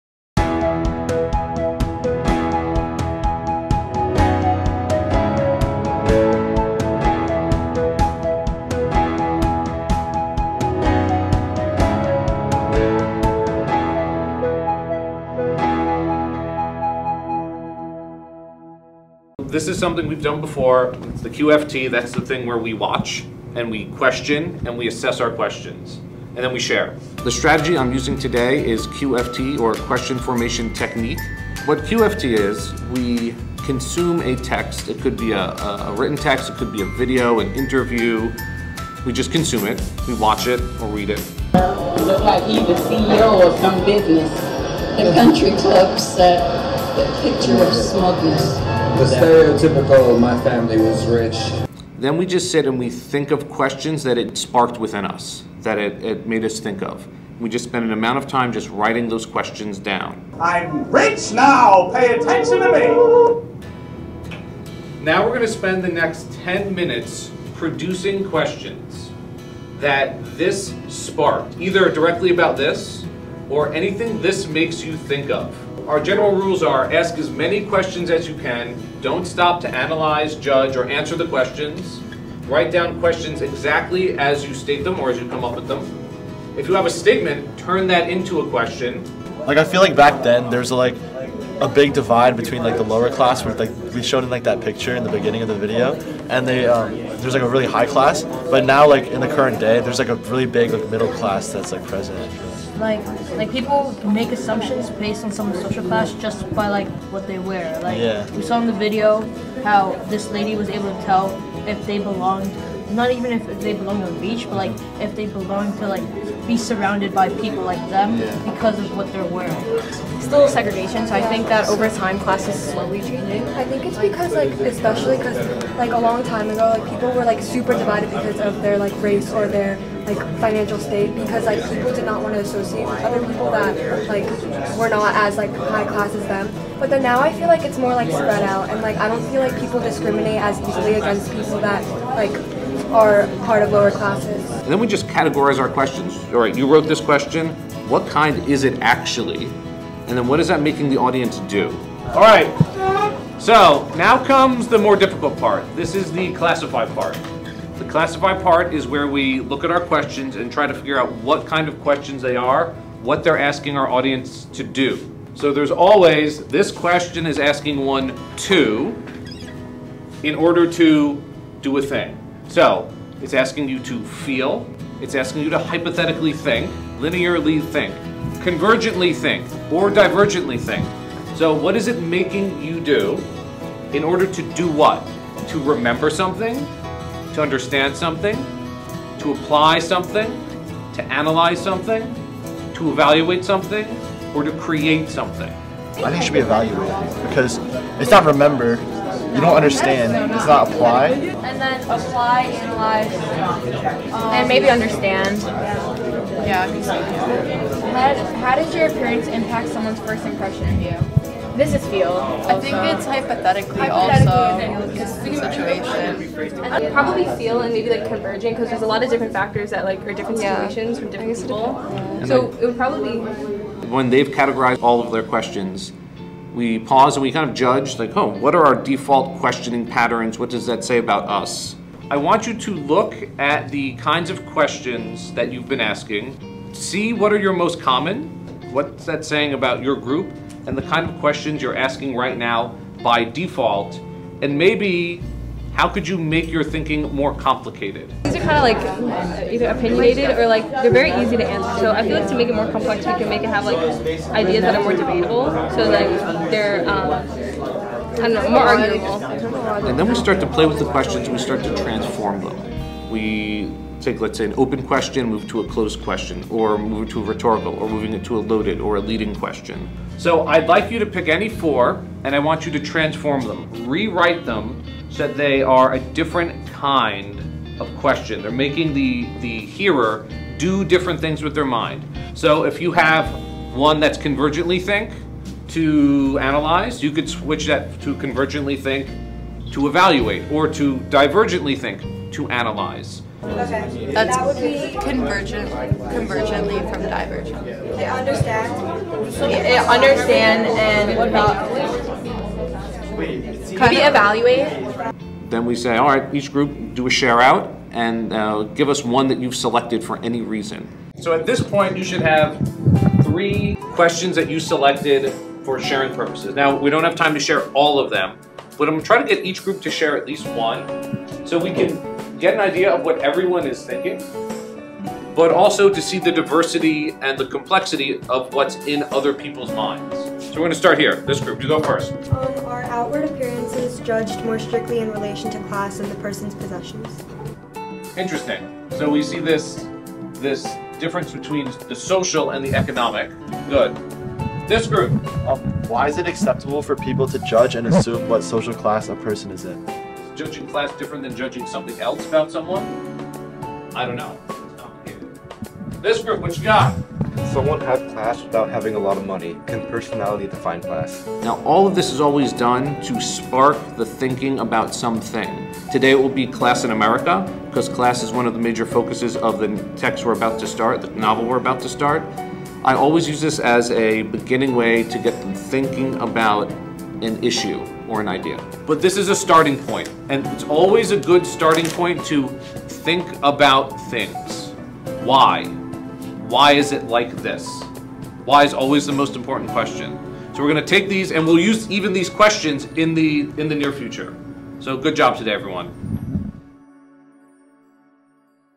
QFT - Question Formation Technique
Carl Buonadonna, English Teacher at BCTS Teterboro, demonstrates the QFT model in which student questioning techniques are developed and enhanced.